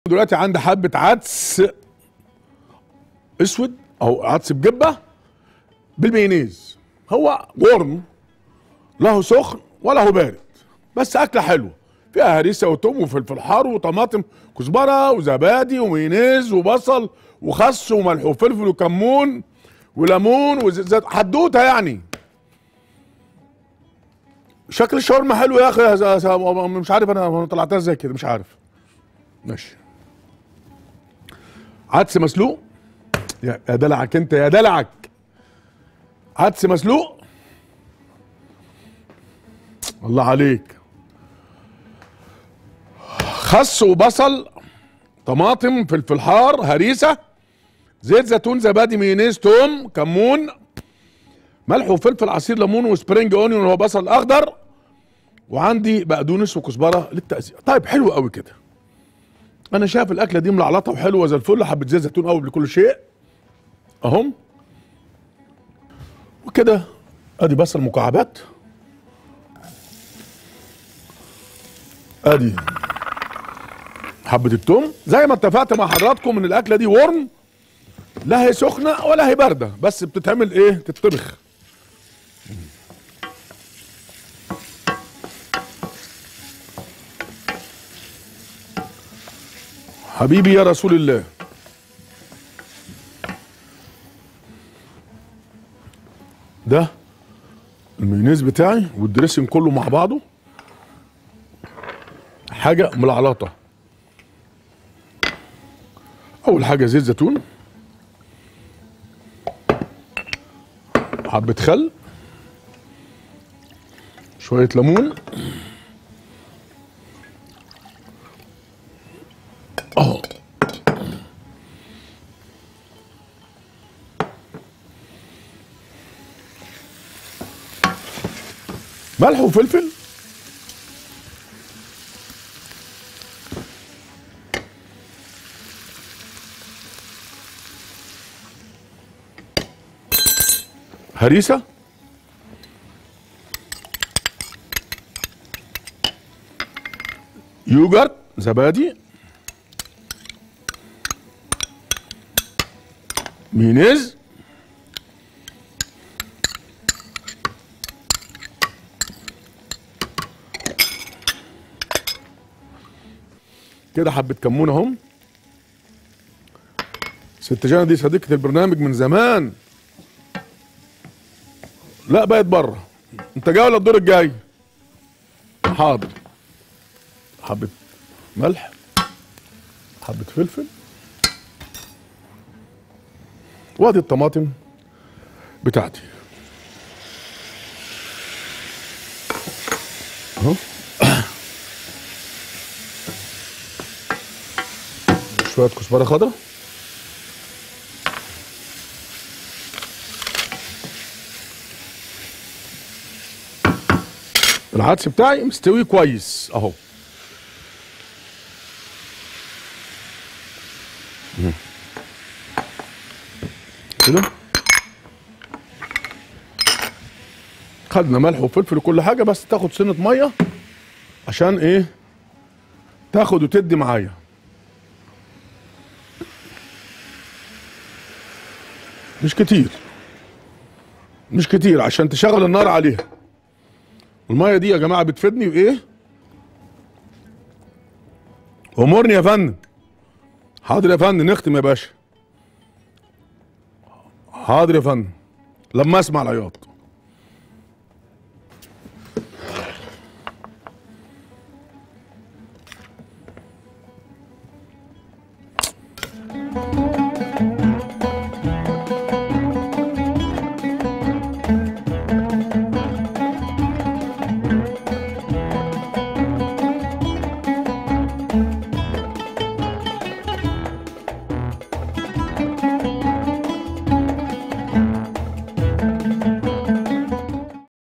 طريقة تحضير عدس بالمايونيز | الشيف شربيني
طريقة عمل عدس بالمايونيز مع الشيف شربيني في الشيف